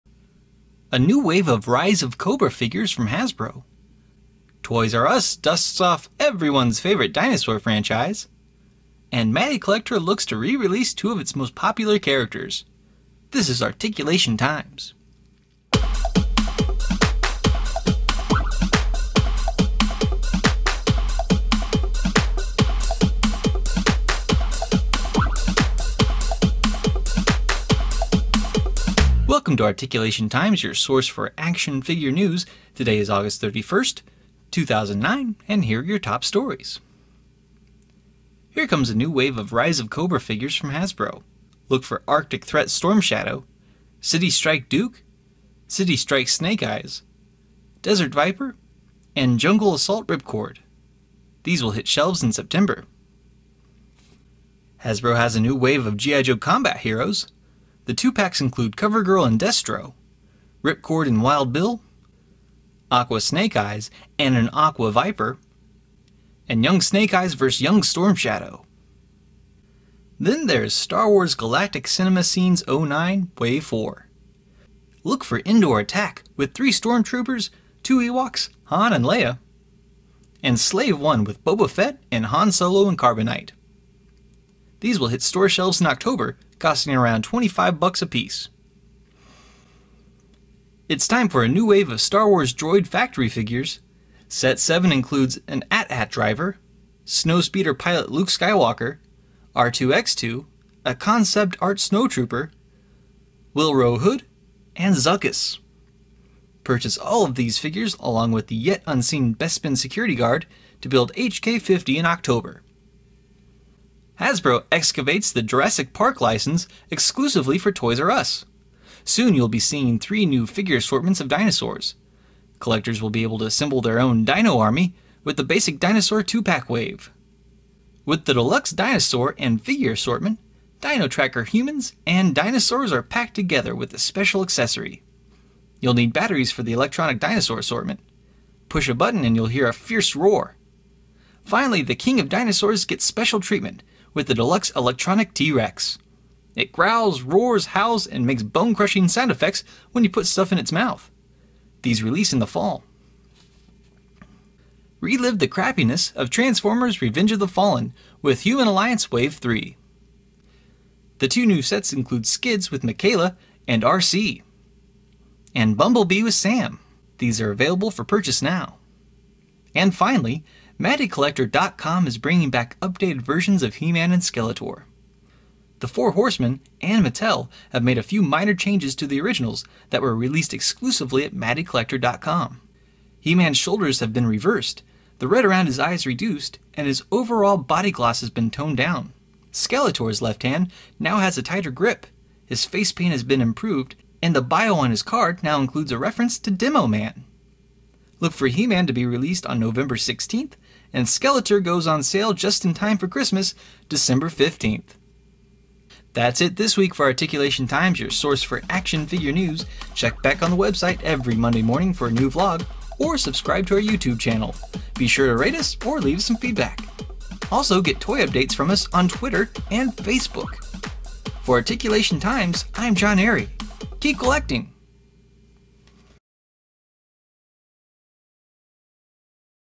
Articulation Times 08-31-09
A new wave of Rise of Cobra figures from Hasbro, Toys R Us dusts off everyones favorite dinosaur franchise and Matty Collector looks to re-release two of its most popular characters.  This is Articulation Times.